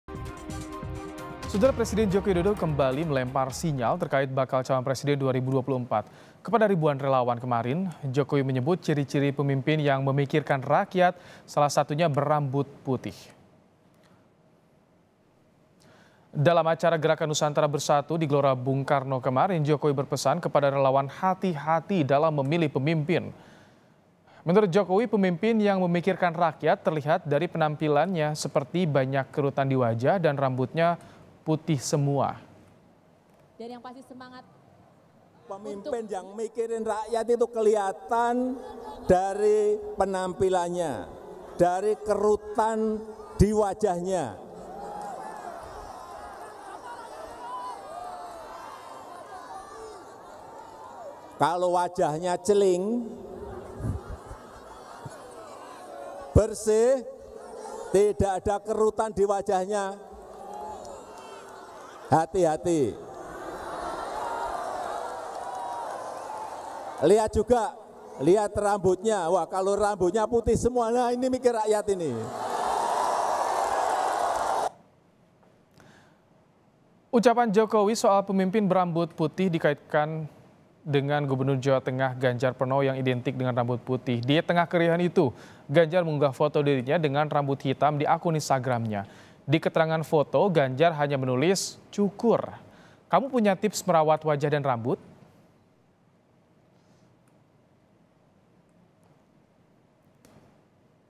Riuh Kode Jokowi soal Pemimpin Rambut Putih, Ganjar Unggah Foto Rambut Hitam
JAKARTA, KOMPAS.TV - Presiden Joko Widodo kembali melempar sinyal terkait bakal calon presiden 2024.

Kepada ribuan relawan, kemarin (26/11/22) Jokowi menyebut ciri-ciri pemimpin yang memikirkan rakyat salah satunya berambut putih.

Baca Juga 4 Alasan Pidato Jokowi kepada Relawan Disebut Simbol Dukungan kepada Ganjar Pranowo di

Dalam acara Gerakan Nusantara Bersatu di Gelora Bung Karno kemarin, Jokowi berpesan kepada relawan hati hati dalam memilih pemimpin.

Menurut Jokowi, pemimpin yang memikirkan rakyat terlihat dari penampilannya seperti banyak kerutan di wajah dan rambutnya putih semua.

Ucapan Jokowi soal pemimpin berambut putih dikait-kaitkan dengan Gubernur Jawa Tengah Ganjar Pranowo yang identik dengan rambut putih.

Di tengah keriuhan itu, Ganjar mengunggah foto dirinya dengan rambut hitam di akun Instagramnya.

Di keterangan foto Ganjar hanya menulis "cukur", kamu punya tips merawat wajah dan rambut?